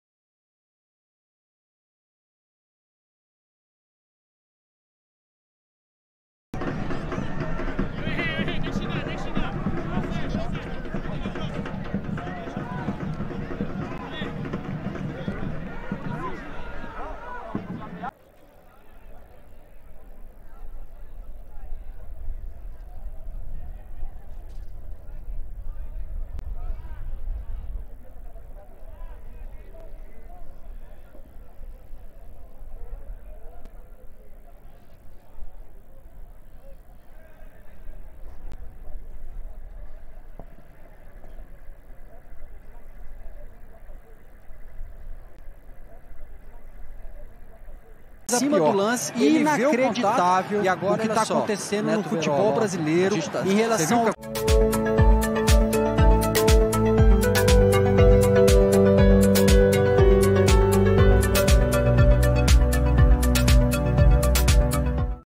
CONFIANÇA É REBAIXADO PRA SÉRIE C / JOGADOR AGRIDE ÁRBITRO (NETO BEROLA CONFIANÇA 0X1 PONTE PRETA)
CONFIANÇA 0X1 PONTE PRETA 20/11/2021. CONFIANÇA PERDEU PRA PONTE PRETA E FOI REBAIXADA PRA SÉRIE C, ALEM DE O JOGADOR DO CONFIANÇA NETO BEROLA APÓS SER EXPULSO AGREDIR O ÁRBITRO. O JOGADOR DO CONFIANÇA NÃO GOSTOU DA EXPULSÃO E AGREDIU O ÁRBITRO. CONFIANÇA 0X1 PONTE PRETA. CONFIANÇA REBAIXADO PRA ´SERIE C